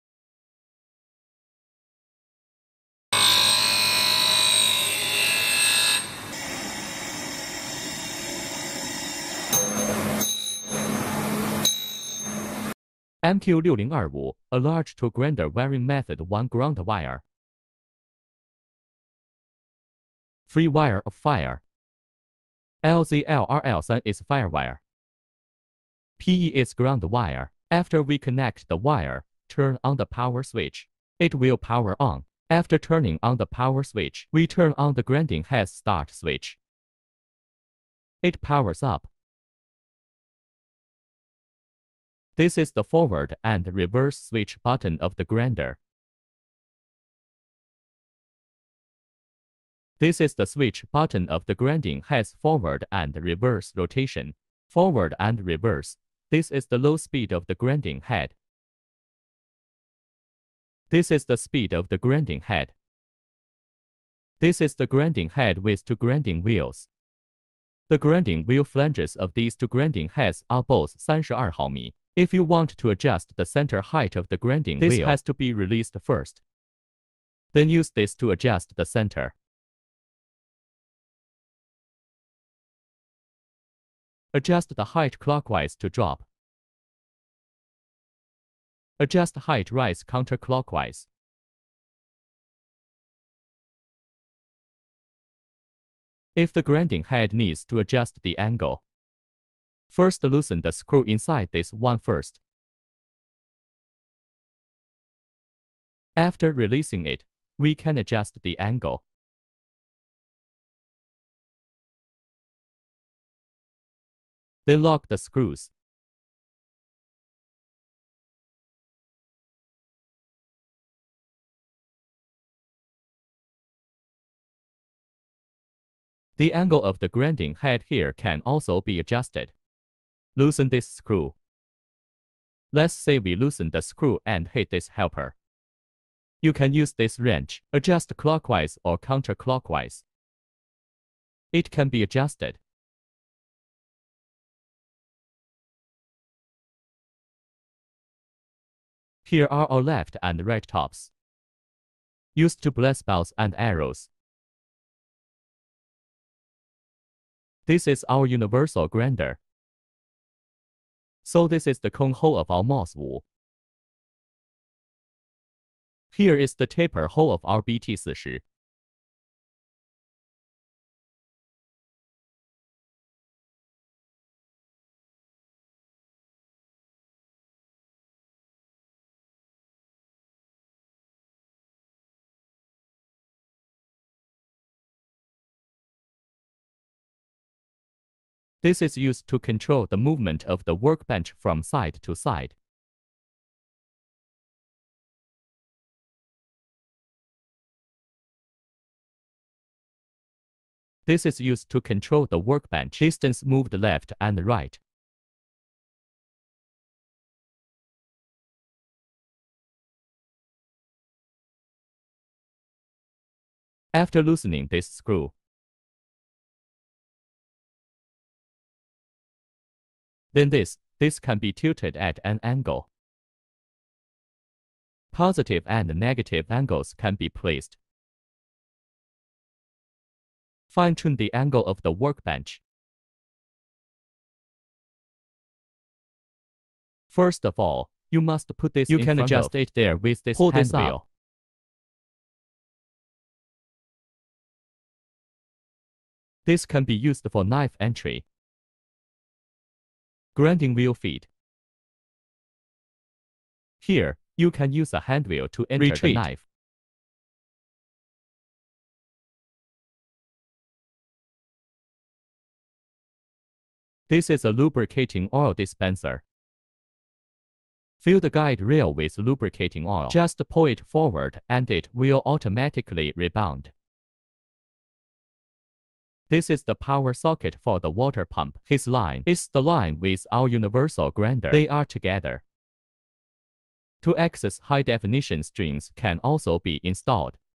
Universal Tool Grinder Machine MQ6025A
This machine is suitable for grinding tools of HSS, tungsten carbide and other materials. It's convenient for cylindrical, surface groove and forming surface grinding work. It can grind various gears, worm wheels hobs, forming cutters, ball end mills, twist drill bits, taper reamer, etc.
If you would like to know more, please send your email to us: